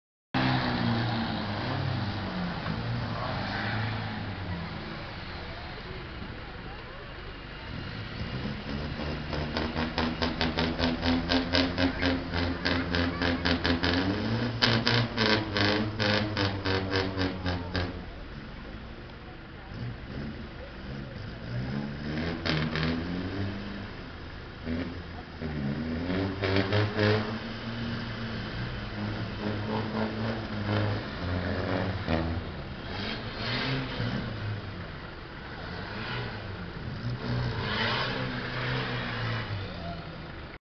Caminhos 4x4 RS
Andando na "cava" de uma olaria...